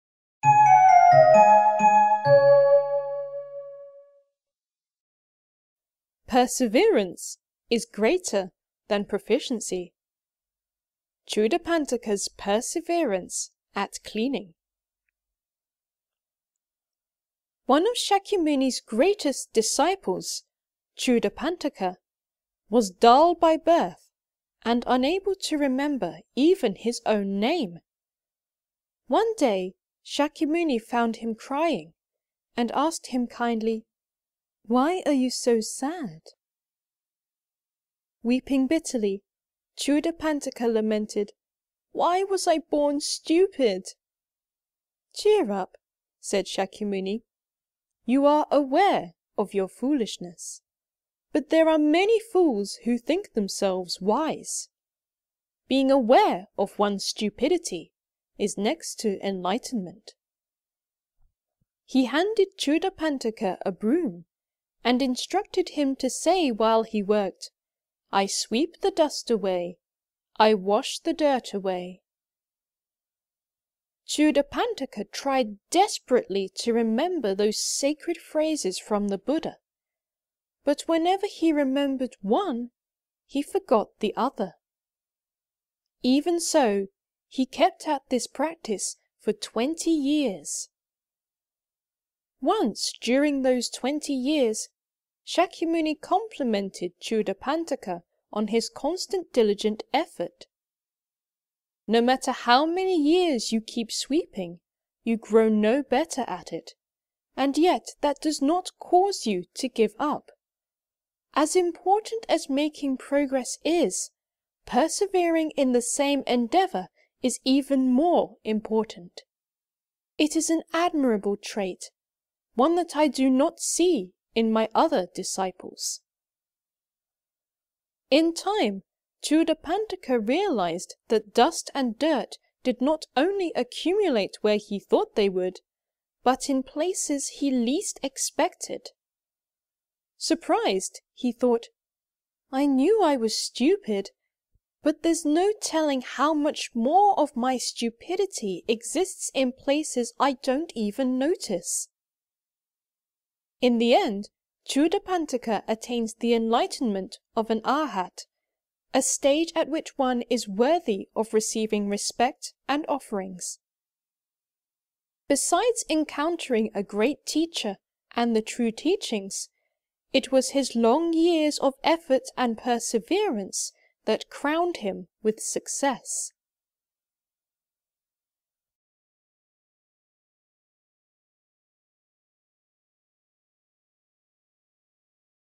Unshakable Spirit Ch. 71 - Perseverance Is Greater than Proficiency
A reading of Chapter 71 from 'Unshakable Spirit' by Kentetsu Takamori.

'Unshakable Spirit' is copyright Ichimannendo Publishing, Inc.